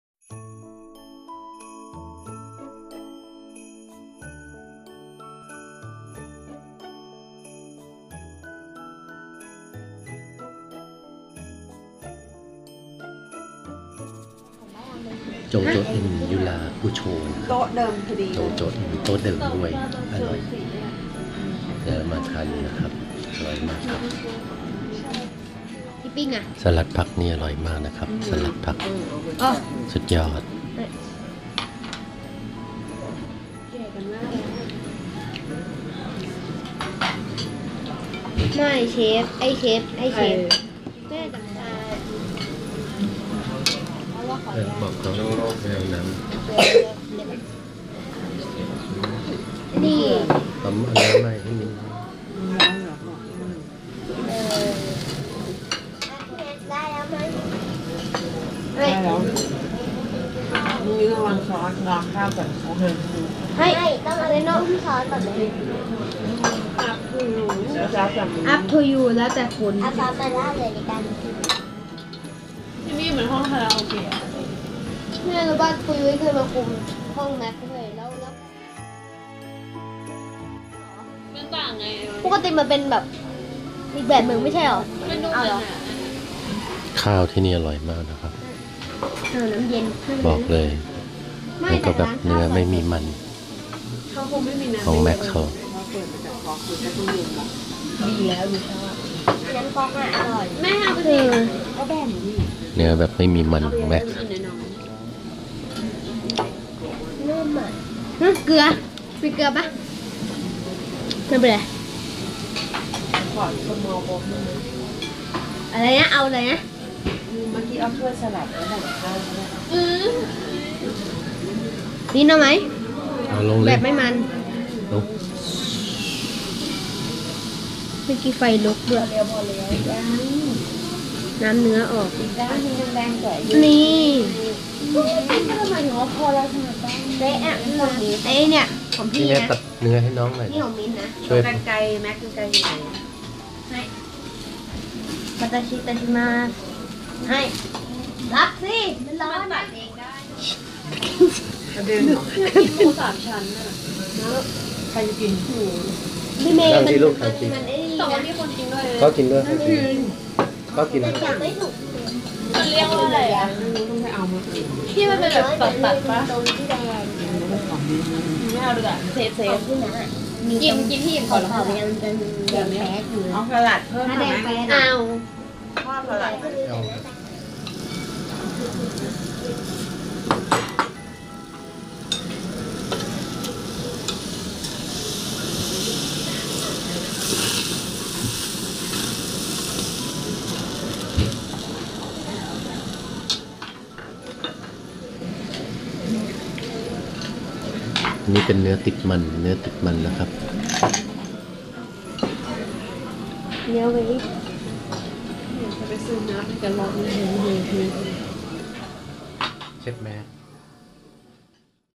JOJOEN YURAKUCHO
สาขานี้ ไม่ค่อยเหม็นควันเนื้อย่างมากเท่าไหร่ครับ อยู่ชั้น 11 ตึก อะไรสักอย่าง ผมมั่วๆไป